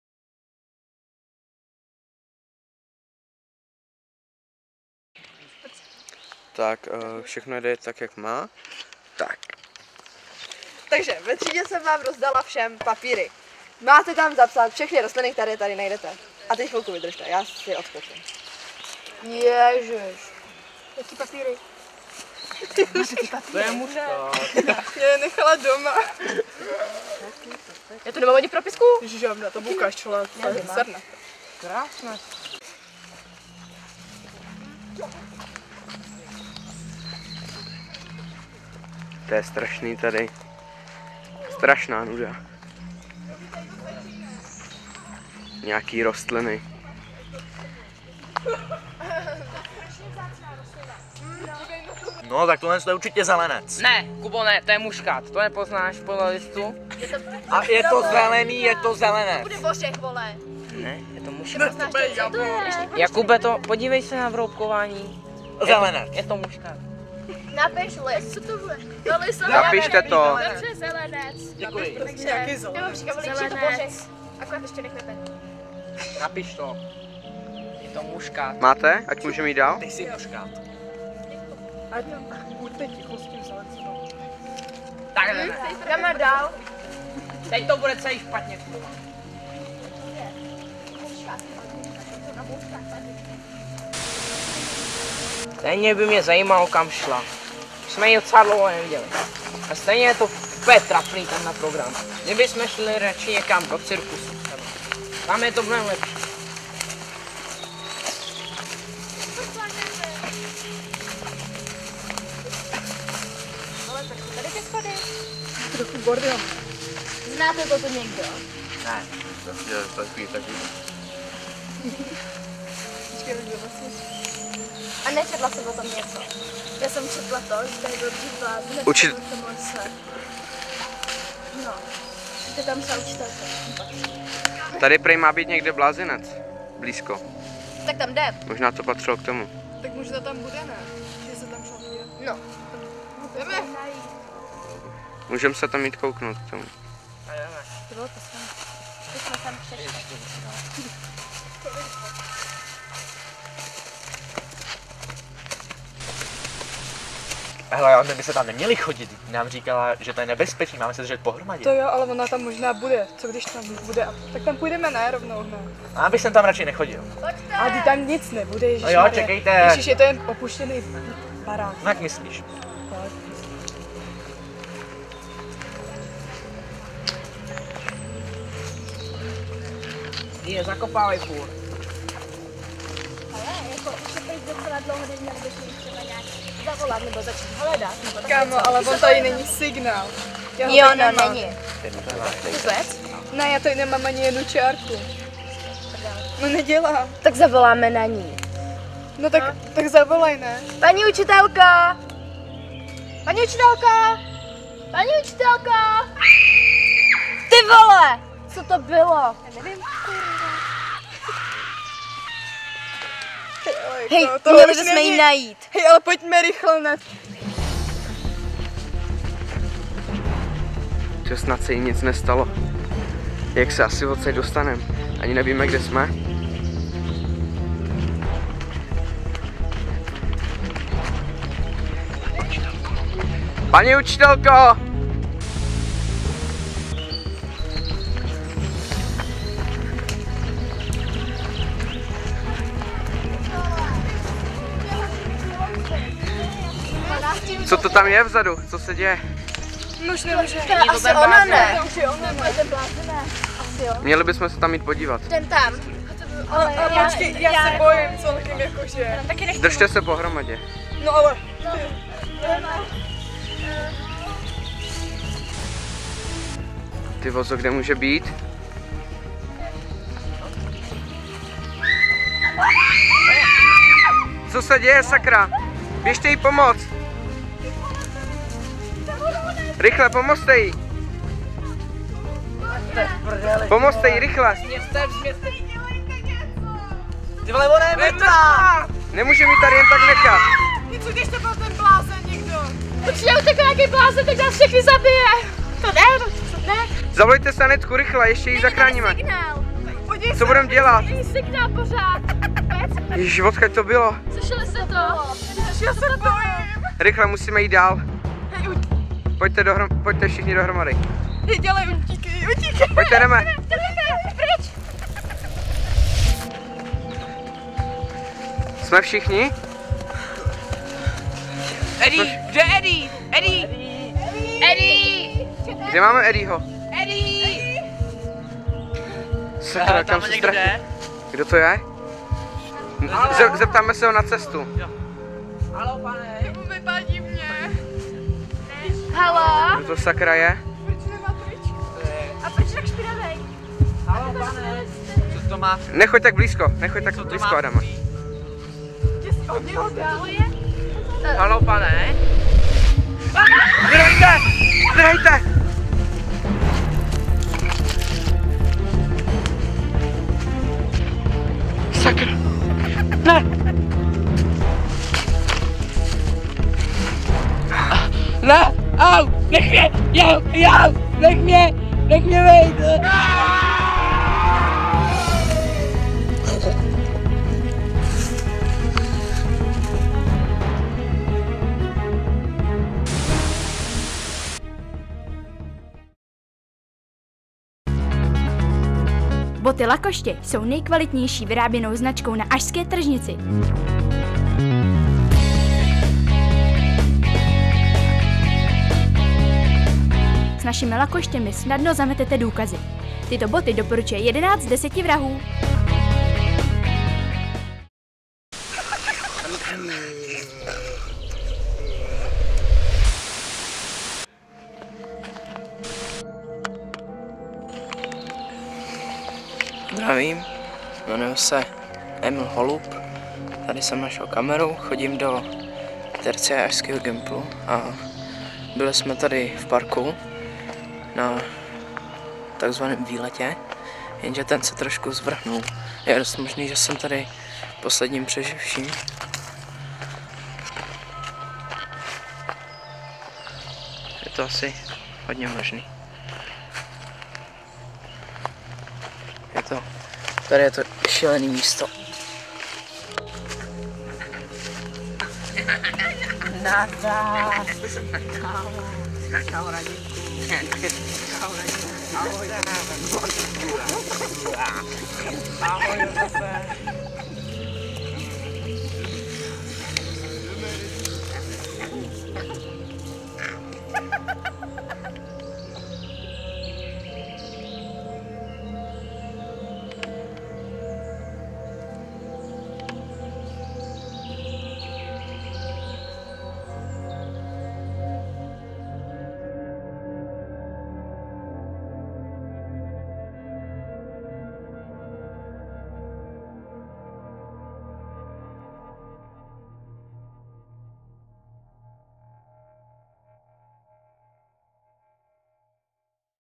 Psychovýlet
V rámci festivalu Ašlerky vytvořili žáci z tercie ašského gymnázia krátký "hororový" film.